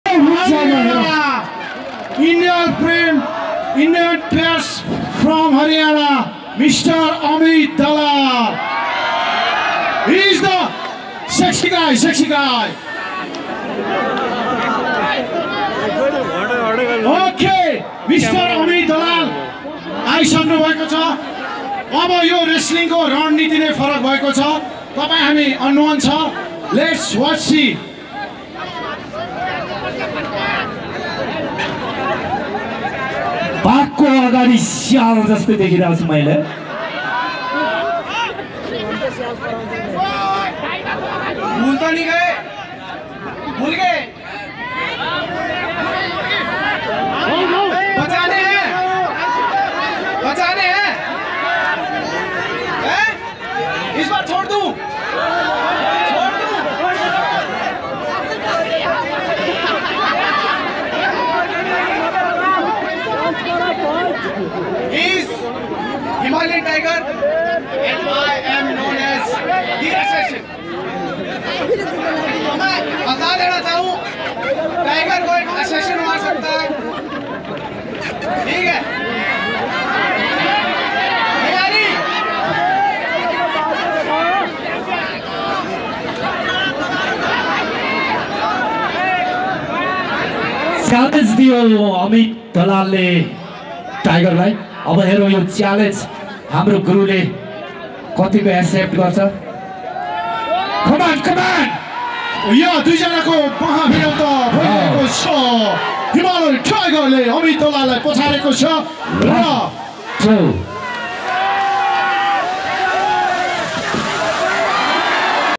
Indian Wrestler Challenges Unbeaten Himalayan Tiger | Knockout in a Single Hit!
In a highly anticipated wrestling showdown, an Indian player steps into the ring to challenge the undefeated Himalayan Tiger from Nepal. However, what unfolds is beyond anyone's expectations as the Indian wrestler gets knocked out with a single devastating hit from the formidable Himalayan Tiger!

Witness the jaw-dropping moment as the Himalayan Tiger showcases his unparalleled strength and skill, leaving spectators in awe of his dominance in the ring. This epic clash serves as a testament to the power and prowess of Nepali wrestling.

Don't miss out on this historic moment in wrestling history.